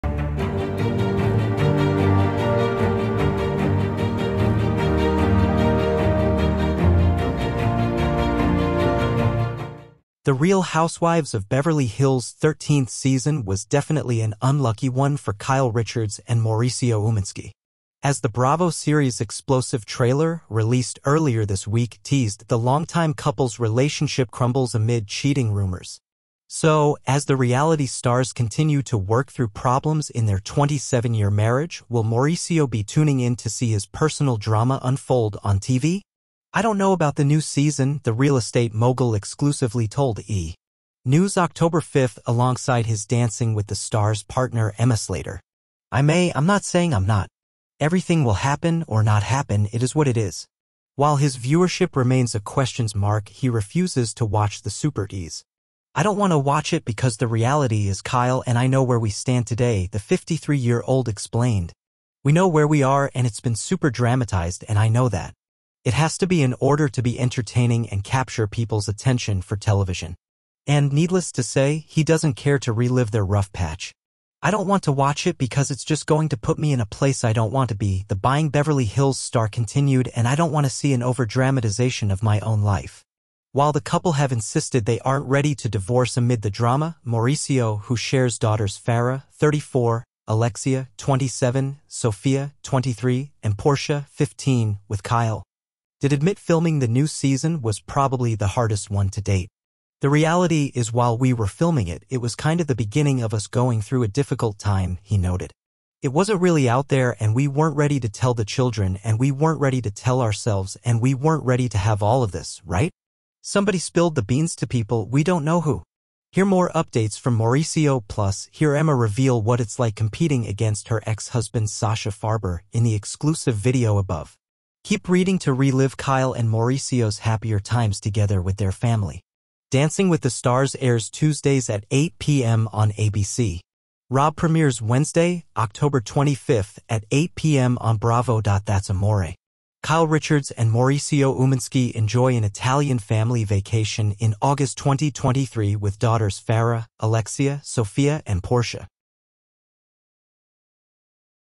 Will Mauricio Umansky Watch Kyle Richards Marriage Troubles Play Out on RHOBH? He Says
The Real Housewives of Beverly Hills' 13th season was definitely an unlucky one for Kyle Richards and Mauricio Umansky.

As the Bravo series' explosive trailer—released earlier this week—teased, the longtime couple's relationship crumbles amid cheating rumors. So, as the reality stars continue to work through problems in their 27-year marriage, will Mauricio be tuning in to see his personal drama unfold on TV?

"I don't know about the new season," the real estate mogul exclusively told E! News Oct. 5 alongside his Dancing With the Stars partner Emma Slater. "I may, I'm not saying I'm not. Everything will happen or not happen, it is what it is."

While his viewership remains a questions mark, he refuses to watch the supertease.

"I don't want to watch it because the reality is Kyle and I know where we stand today," the 53-year-old explained. "We know where we are and it's been super dramatized and I know that. It has to be in order to be entertaining and capture people's attention for television."